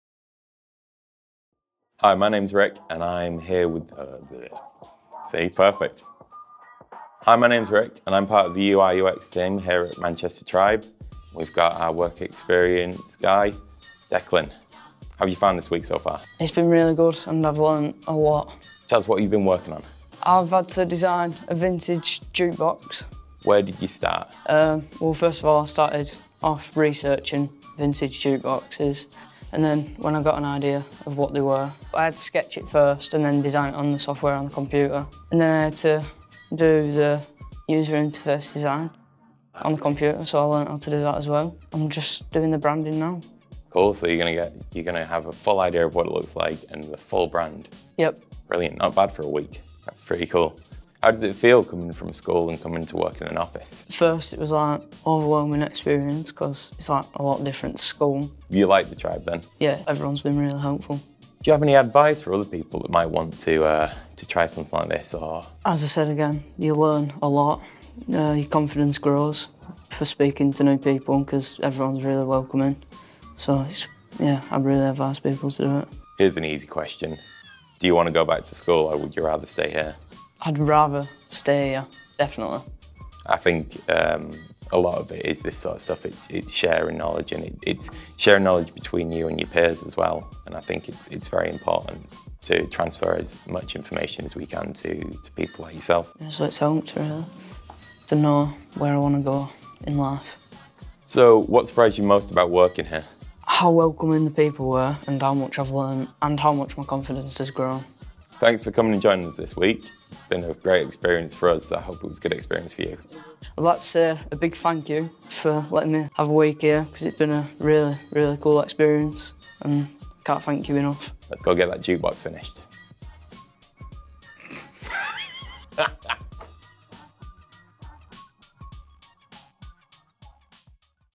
An Academy Experience
Declan talks about his work experience at Midas, Manchester.